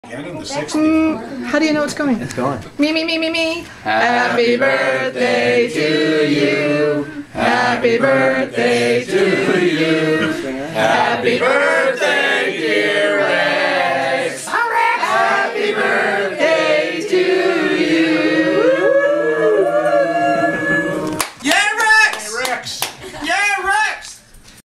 Happy Birthday Rex!
A family birthday wish for Rex Hill